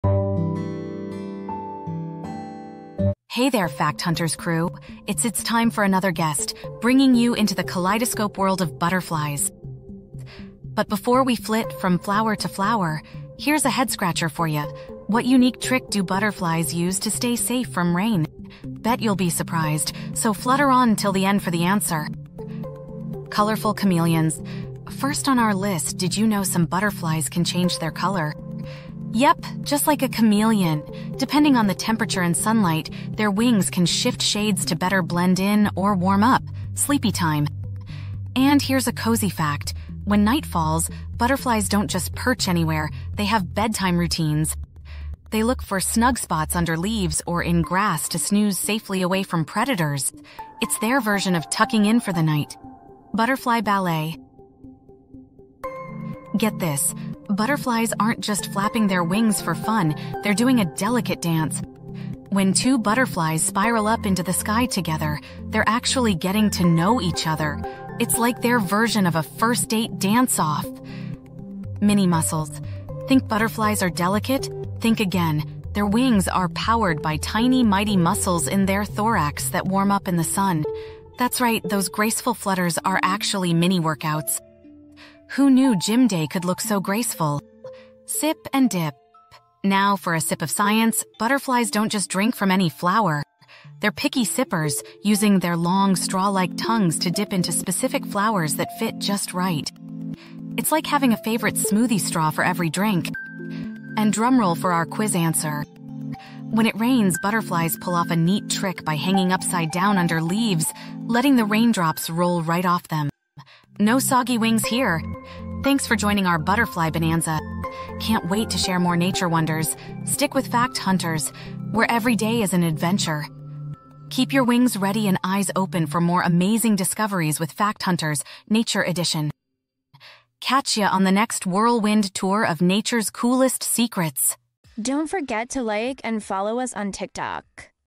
Color-Changing Butterflies: Nature's Chameleons
Get ready to be amazed by nature's chameleons: color-changing butterflies! 🌈🦋 Join us, Fact Hunters crew, as we dive into the vibrant world of these incredible creatures. Discover how some butterflies magically shift their hues in response to sunlight and temperature, blending seamlessly into their surroundings. Did you know they have bedtime routines too? Prepare to have your mind blown as we unravel their nightly search for safety, and their delicate dance during a skyward spiral that’s like a first date! 💃

These mini muscle marvels aren't just about elegance—they're powered by mighty thorax muscles, turning every flutter into a workout. And when it rains, butterflies ingeniously hang upside down to keep their wings dry. 😮 Don't miss out on this whirlwind tour of animal behavior and wildlife wonders! 🌿✨

CHAPTERS:
00:00 - Intro
00:22 - Chameleons: Color Change, Adaptation, Camouflage
00:40 - Bedtime Routines: Sleep Tips, Healthy Habits, Nighttime Rituals
00:55 - Butterfly Ballet: Dance, Movement, Nature
01:12 - Mini Muscles: Exercise, Strength, Kids Fitness
01:30 - Picky Sippers: Healthy Drinks, Nutrition, Kid-Friendly
01:46 - Rainy Day Trick: Indoor Activities, Fun Games, Creativity
01:56 - Outro